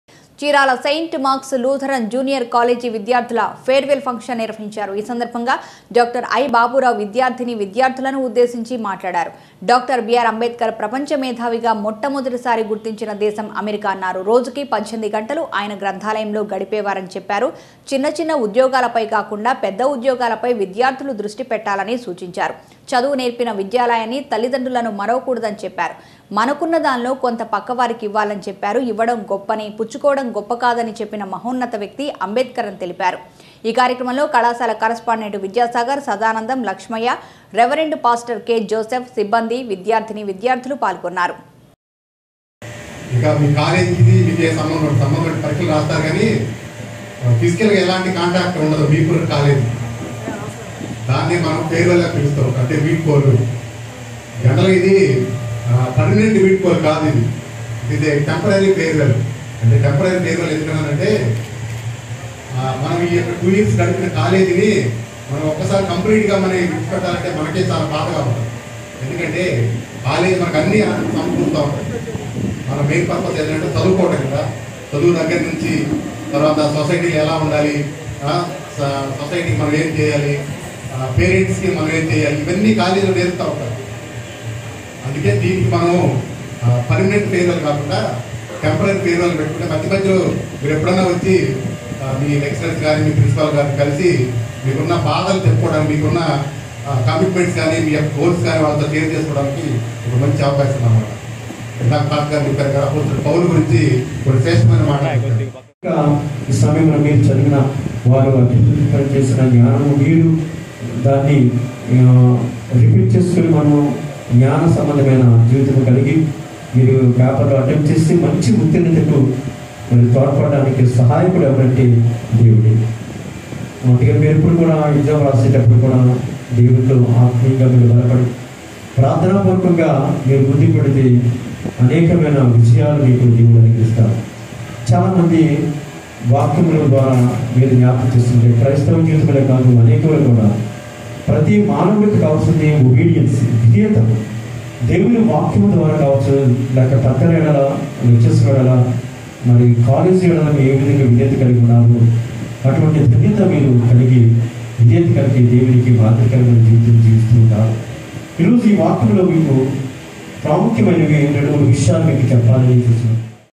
సెయింట్ మార్క్స్‌ లూధరన్‌ కళాశాల విద్యార్ధుల వీడ్కోలు సభలో డాక్టర్‌ బాబురావు || DN5 News
చీరాల సెయింట్‌ మార్క్స్ లూథర్న్ జూనియర్ కాలేజీ విద్యార్ధల ఫేర్ వెల్ ఫంక్షన్‌ నిర్వహించారు. ఈసందర్భంగా డాక్టర్ ఐ బాబురావు విద్యార్థిని విద్యార్థులను ఉద్దేశించి మాట్లాడారు. డాక్టర్ బీఆర్ అంబేద్కర్ ప్రపంచ మేధావిగా మొట్టమొదటిసారి గుర్తించిన దేశం అమెరికా అన్నారు. రోజుకి 18గంటలు ఆయన గ్రంధాలయంలో గడిపేవారని చెప్పారు. చిన్న చిన్న ఉద్యోగాలపై కాకుండా పెద్ద ఉద్యోగాలపై విద్యార్ధులు దృష్టి పెట్టాలని సూచించారు. చదువు నేర్పిన విద్యాలయాన్ని, తల్లి తండ్రులను మరువకూడదని చెప్పారు. మనకున్న దానిలో కొంత ప్రక్కవారికి ఇవ్వాలని చెప్పారు. ఇవ్వడం గొప్పని పుచ్చుకోవడం గొప్పకాదని చెప్పిన మహోన్నతవ్యక్తి అంబేద్కరని తెలిపారు. కార్యక్రమంలో కళాశాల కరెస్పాండెంట్ విద్యాసాగర్, సదానందం, లక్ష్మయ్య, రెవరెండ్ పాస్టర్ కె జోసెఫ్, సిబ్బంది, విద్యార్థిని విద్యార్థులు పాల్గొన్నారు.